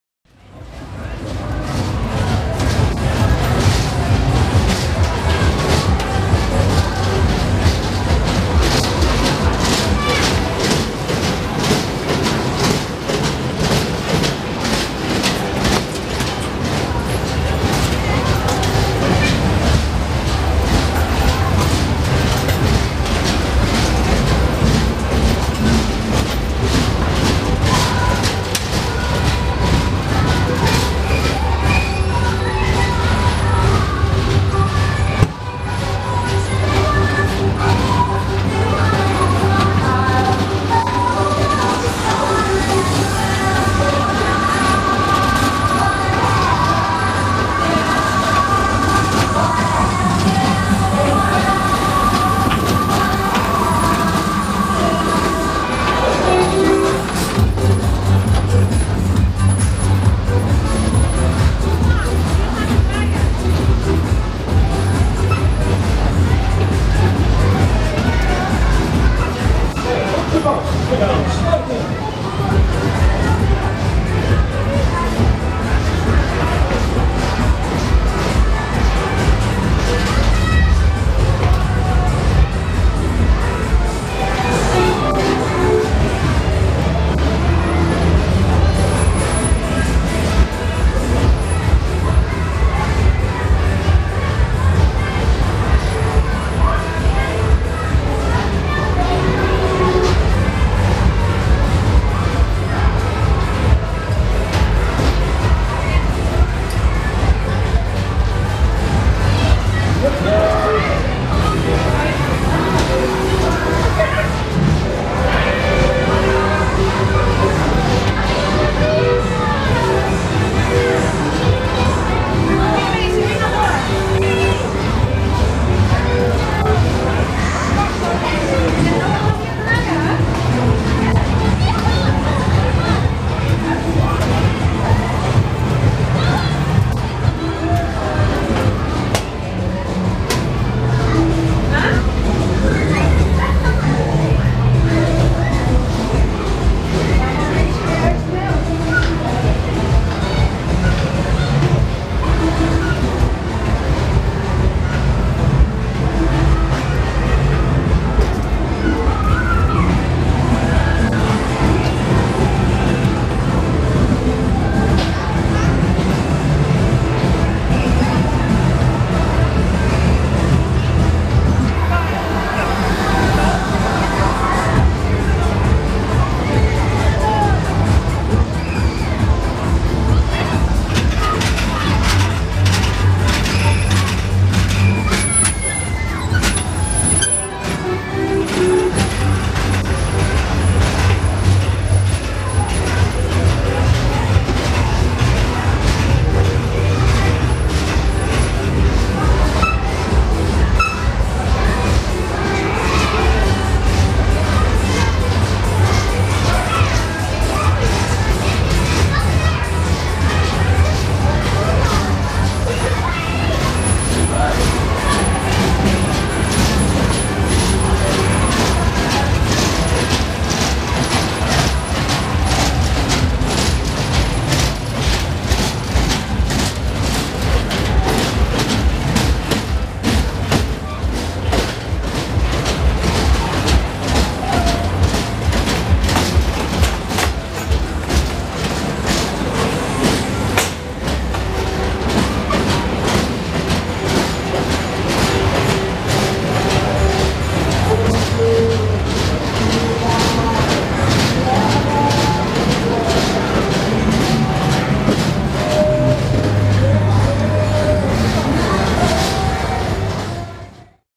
Onride| Happy Farm (M.J V/D Wiel) Voorjaarskermis Eibergen 2016
Onride Happy Farm Funhouse van kermisexploitant/Schausteller M.J V/D Wiel op de Voorjaarskermis Eibergen 2016!